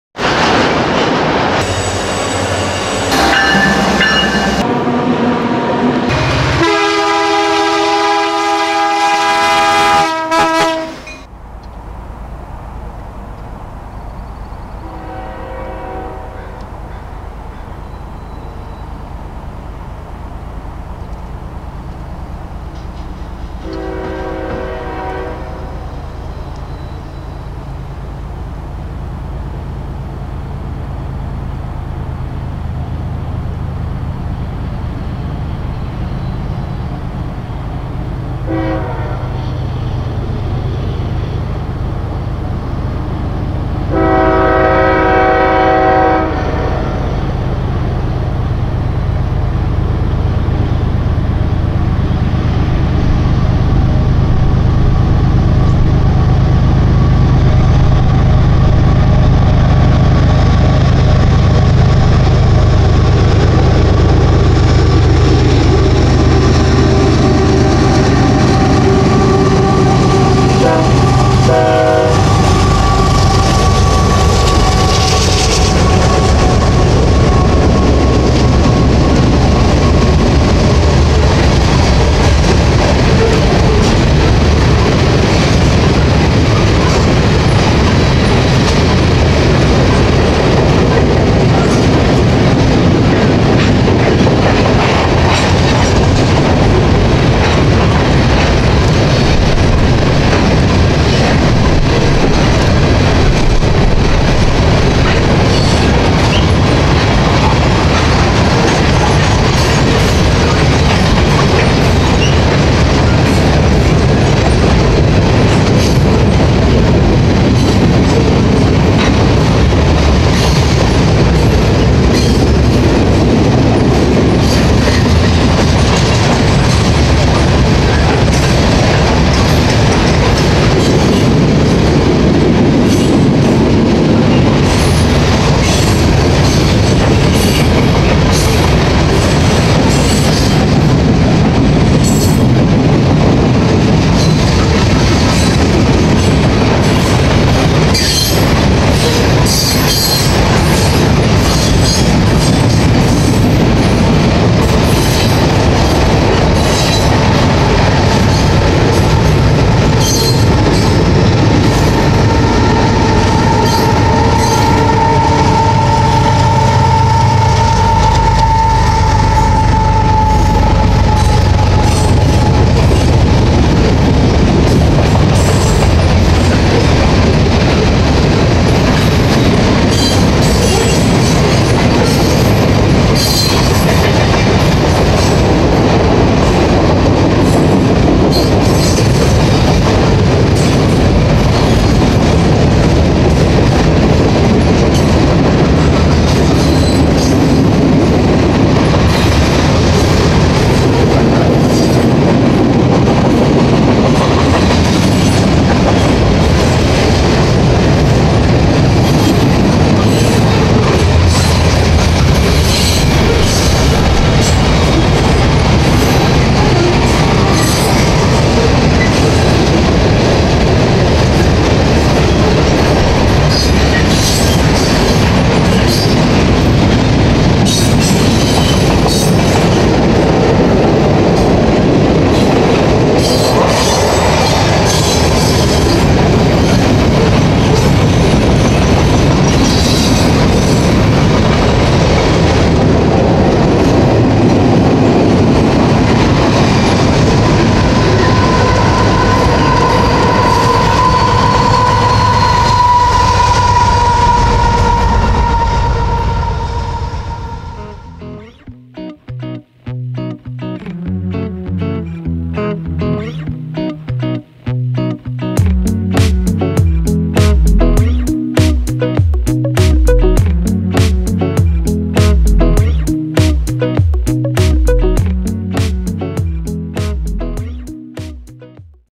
CP 8642 Ex Holiday Unit Brings up the Middle of CP 603!
Hey everyone! After seeing CP 300 minutes before, I could hear that 603 was on their way! Soon the heavy potash train came honking their way over the bridge with a friendly crew on board! Also on the train was a nice surprise in the middle, with CP ex-Holiday Unit 8642! Although we had planned on stopping in Cochrane on the way back, several circumstances didn't let it happen, therefore this was the last train that we would see on our 2-day trip.

I'd like to give a HUGE thank you to Lucas and Hugo, who helped me so much on this trip they helped in every way from giving me spots to go to, identifying the trains I saw, and even gave me heads up on trains that were coming my way!

Locomotives:
CP AC4400CW 8642 (Ex. Holiday Unit)(Mid-DPU)
CP ES44AC 8933 (Mid-DPU)
CP AC4400CWM 8047 (End-DPU)

Equipment used:
Video camera: Canon VIXIA HF R80
Photo Camera: Canon EOS Rebel XS
Lens: Canon 18-55mm
Mic: Takstar SGC 598
Tripod: Amazon Basics 60”

Filmed and photographed by Alberta Bound Railfanning on June 28th, 2020, at 1:59 PM MDT